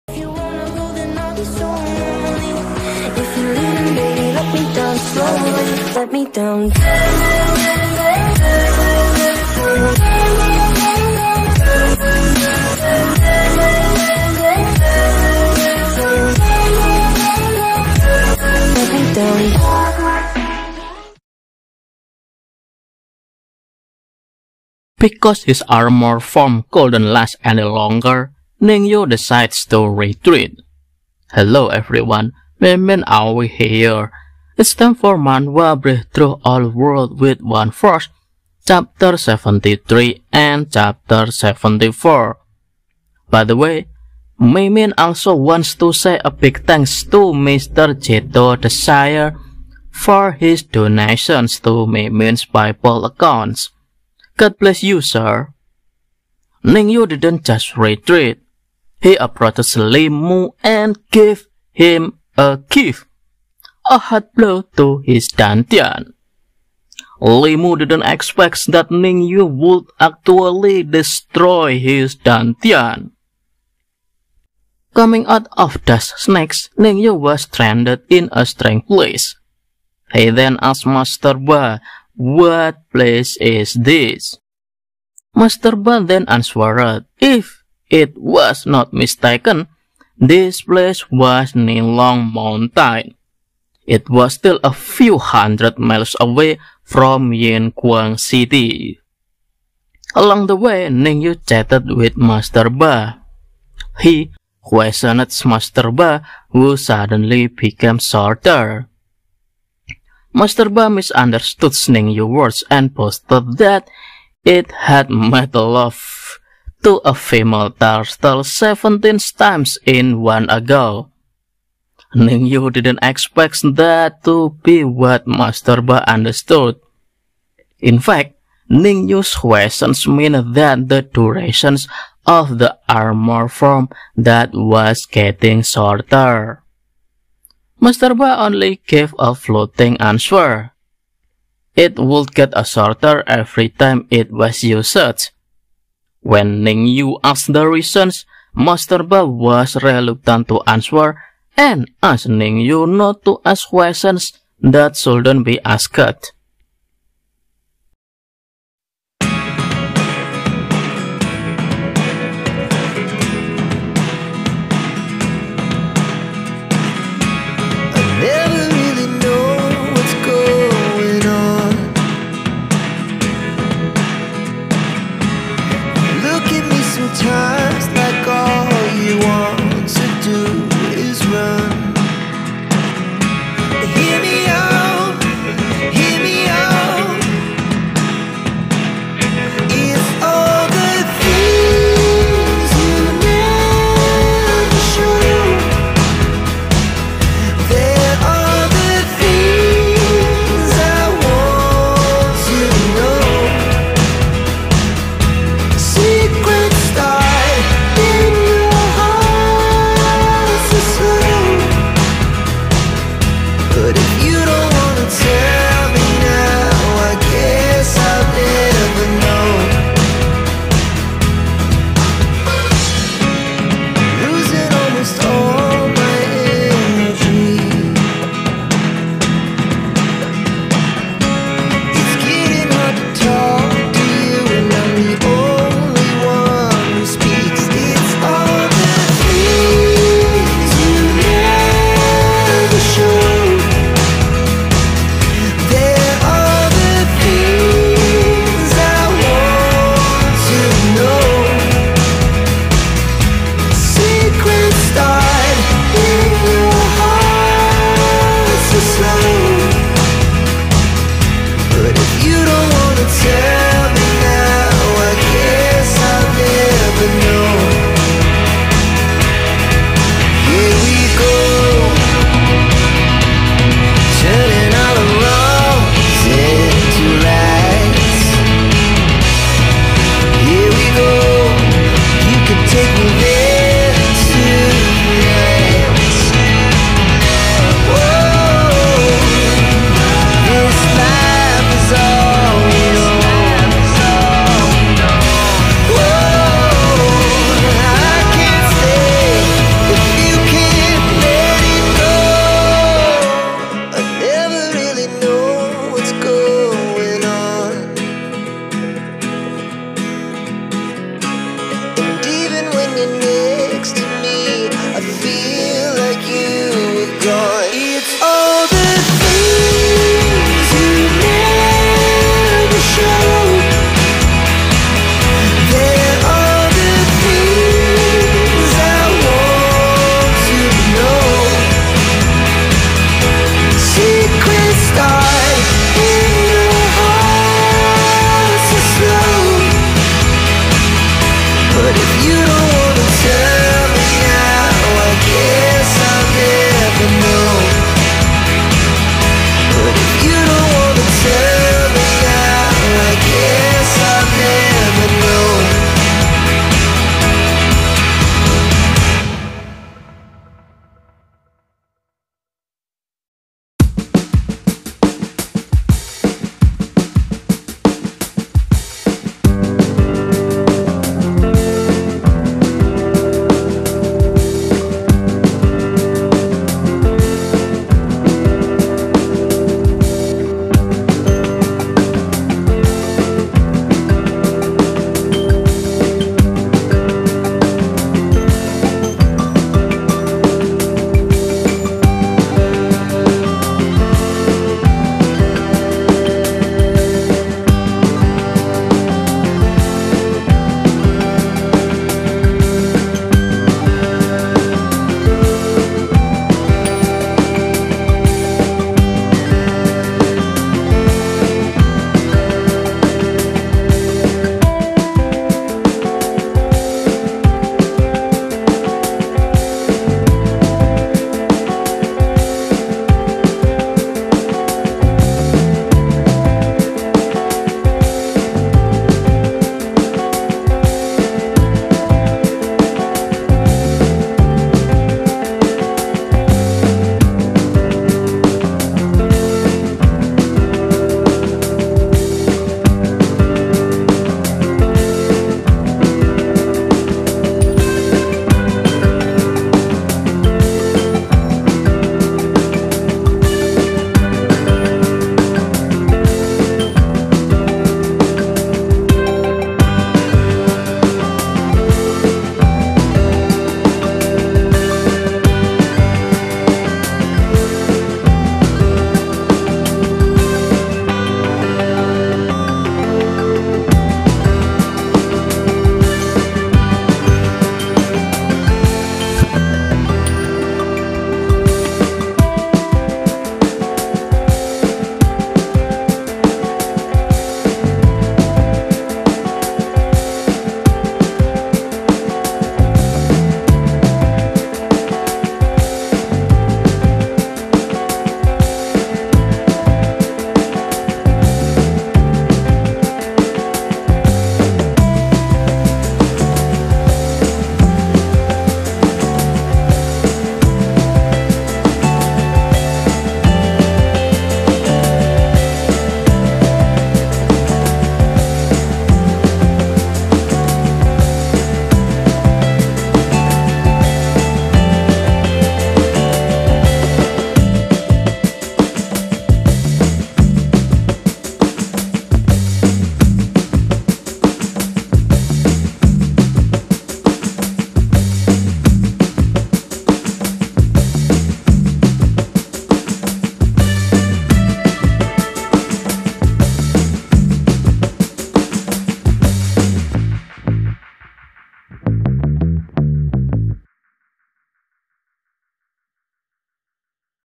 Body Of Ten Thousands Beasts ™ Breakthrough All Worlds With One Force Chapter 73-74
A long trip to Yinguang City, Master Ba teach Ning Yu Ten Thousands Beast Body Art. Technic that made Ning Yu body, stronger than beast body

 COPYRIGHT / LICENSING